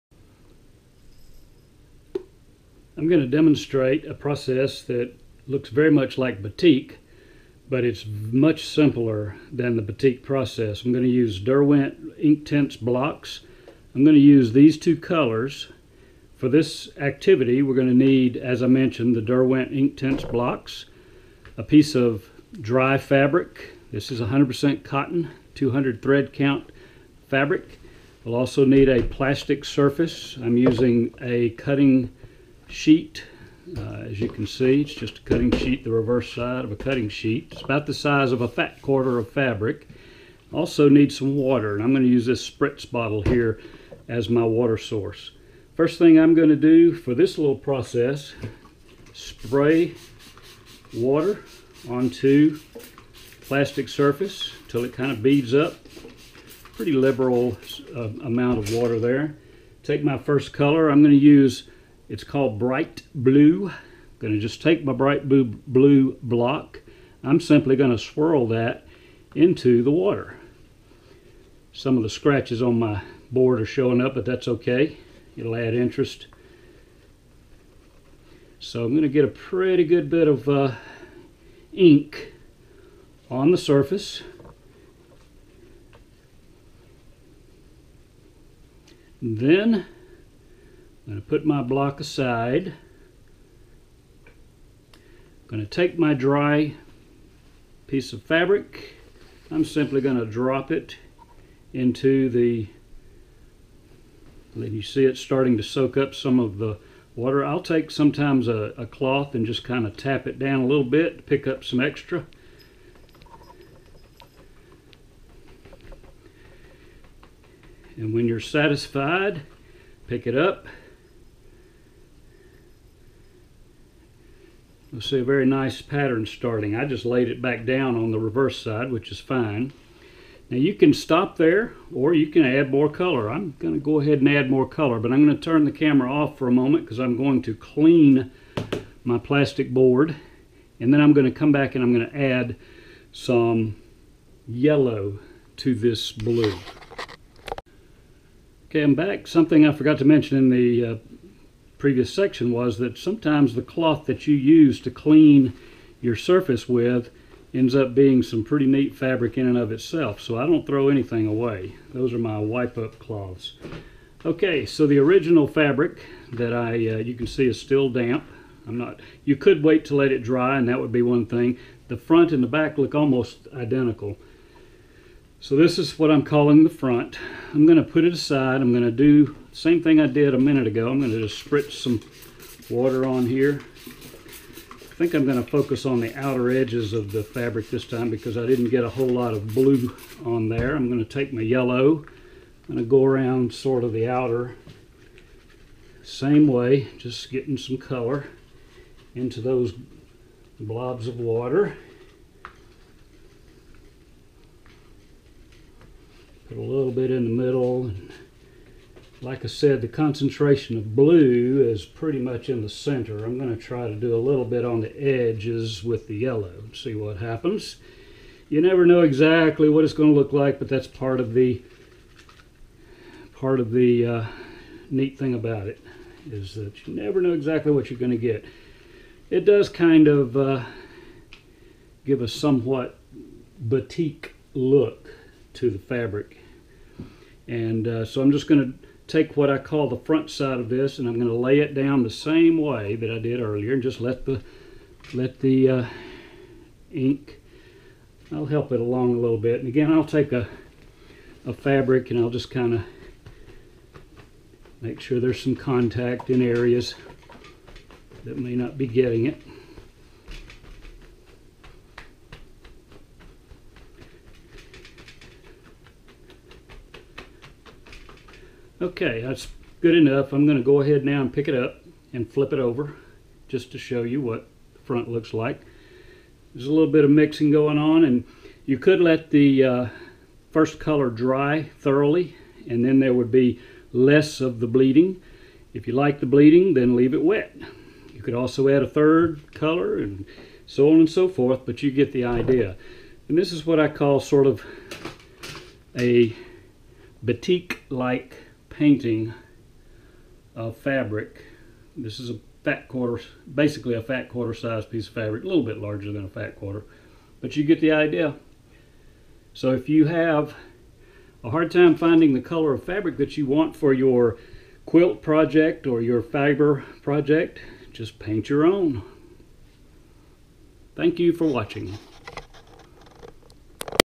DIY How To Paint Fabric To Create Random Batik Like Designs
Use Inktense to paint/dye fabric to create random designs that look similar to batik.

Inktense Blocks